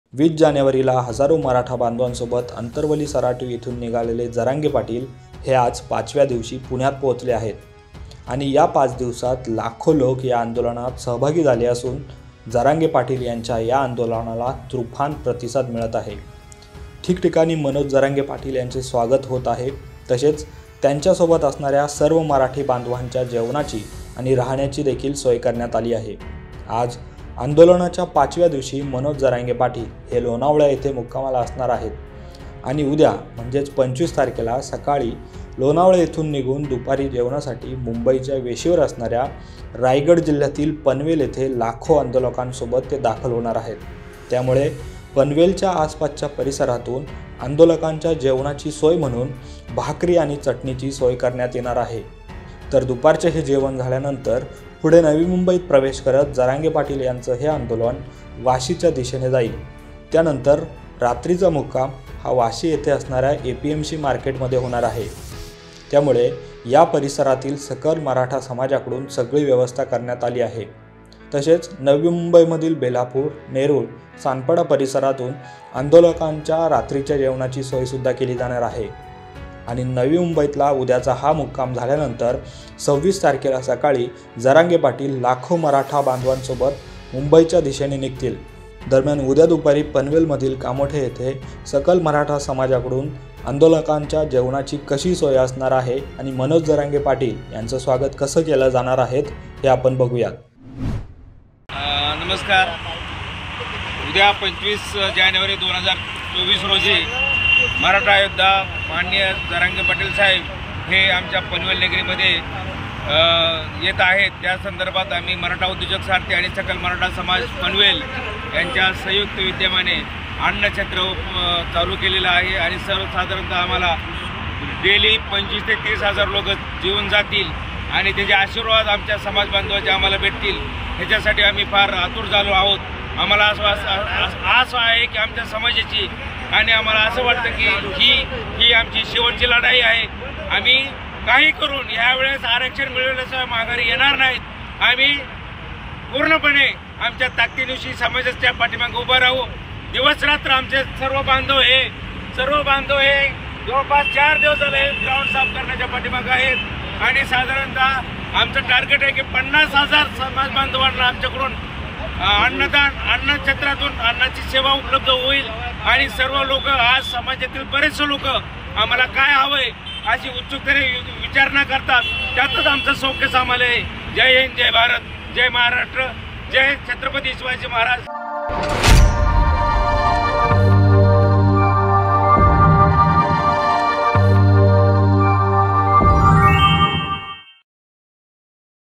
Manoj Jaranage Rally: मनोज जरांगे पाटील उद्या मुंबईच्या वेशीवर,नवी मुंबईत अस केलं जाणार स्वागत
मराठा समाजाला आरक्षण मिळावं म्हणून पुन्हा एकदा आंदोलन उभं करणारे मनोज जरांगे-पाटील हे 26 तारखेला मुंबईत पोहचणार आहेत.20 जानेवारीला हजारो मराठा बांधवासोबत अंतरवली सराटी येथून निघालेले मनोज जरांगे पाटील हे आज पाचव्या दिवशी पुण्यात पोहचले आहेत.आणि या पाच दिवसात लाखो लोक या आंदोलनात सहभागी झाले असून जरांगे पाटील यांच्या या आंदोलनाला तुफान प्रतिसाद मिळत आहे.नवी मुंबईत मनोज जरांगे यांच्या आंदोलनाच नियोजन कस असणार आहे जाणून घेऊ.